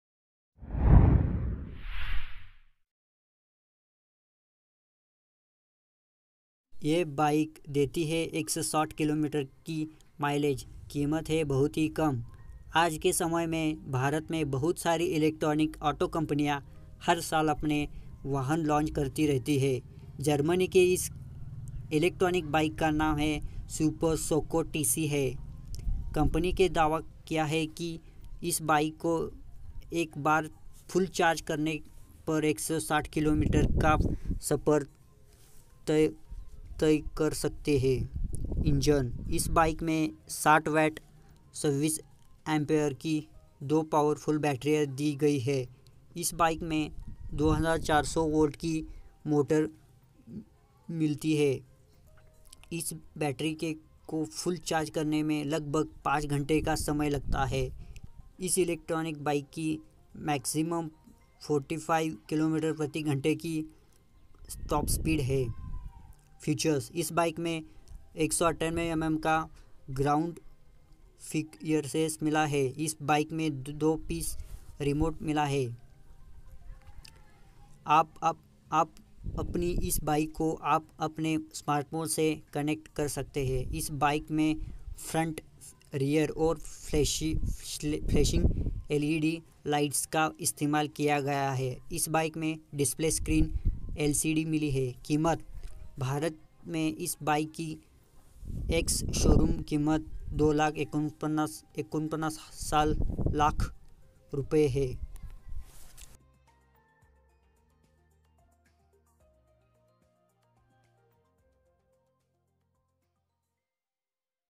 यह बाइक देती है 160 किलोमीटर की माइलेज, कीमत है बहुत ही कम.
आज के समय में भारत में बहुत सारी इलेक्ट्रिक ऑटो कंपनियां हर साल अपने वाहन लॉन्च करती रहती हैं। जर्मनी की इस इलेक्ट्रिक बाइक का नाम Super Soco TC है। कंपनी ने दावा किया है, कि इस बाइक को एक बार फुल चार्ज करने पर 160 किलोमीटर का सफर तय कर सकती है।

इंजन:-

इस बाइक में 60V 26Ah की दो पावरफुल बैटरी दी गई है। इस बाइक में 2400 वोल्ट की मोटर मिलती है। इस बैटरी को फुल चार्ज करने में लगभग 5 घंटे का समय लगता है। इस इलेक्ट्रिक बाइक की मैक्सिमम 45 किलोमीटर प्रति घंटे की टॉप स्पीड है।

फीचर्स:-

इस बाइक में 198mm का ग्राउंड क्लियरेंस मिला है। इस बाइक में 2-पीस रिमोट मिला है। आप अपनी इस बाइक को आप अपने स्मार्टफोन से कनेक्ट कर सकते हैं। इस बाइक के फ्रंट, रियर और फ्लैशिंग एलईडी लाइट्स का इस्तेमाल किया गया है। इस बाइक में डिस्प्ले स्क्रीन एलसीडी मिली है।
कीमत:

भारत में इस बाइक की एक्स शोरूम कीमत 2.49 लाख रुपए है।

It's FREE...